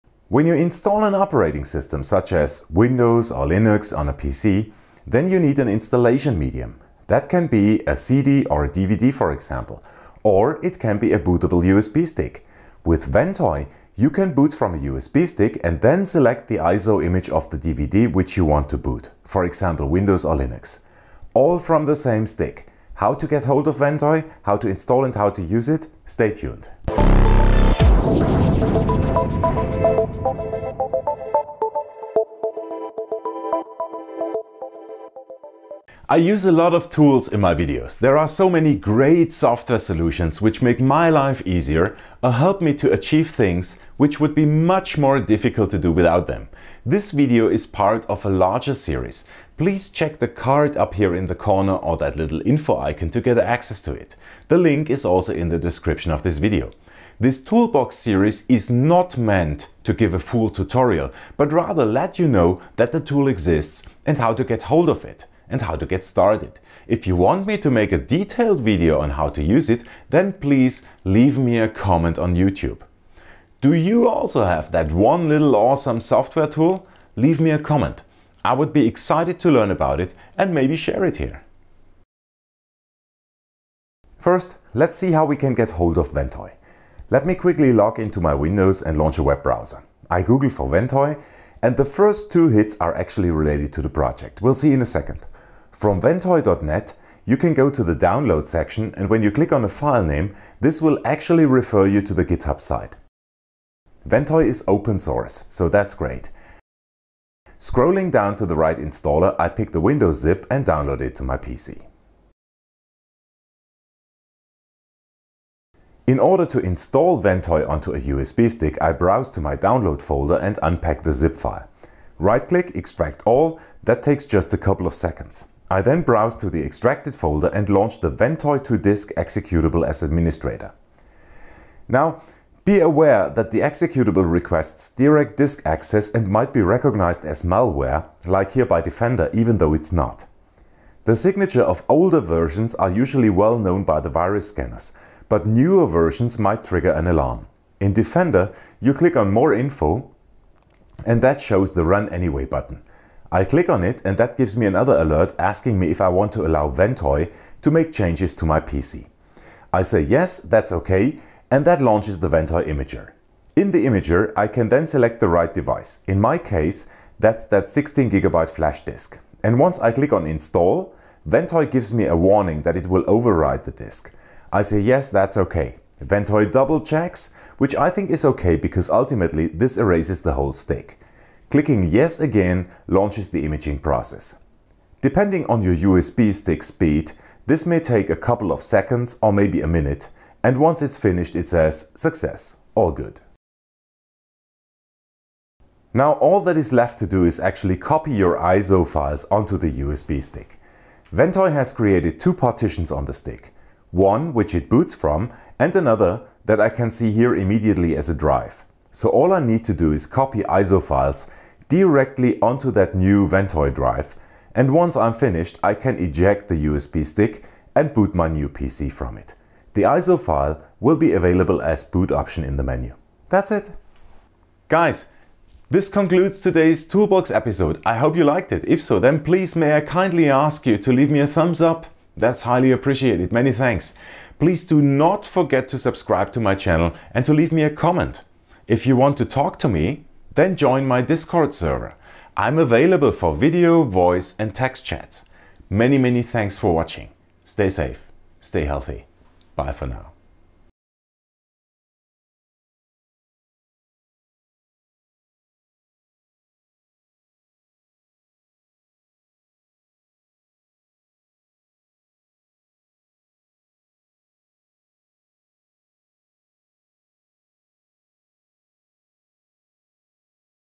How to make a USB boot stick with multiple ISOs on it (Marcs Toolbox)
Let's make a bootable USB stick from which you can boot Windows from USB or boot Linux from USB - both from the same stick !!! First episode of my new Toolbox series. Boot multiple isos from one usb with Ventoy. Gone are the times where we needed to create USB Sticks with Etcher or DD ! Ventoy is a real Etcher alternative but also an Easy2Boot alternative .

Want to talk to me?

0:00 Teaser
0:27 Jingle
0:35 Toolbox Intro
1:22 How to get Ventoy?
1:53 How to install Ventoy?
3:13 How to use Ventoy?
3:43 outro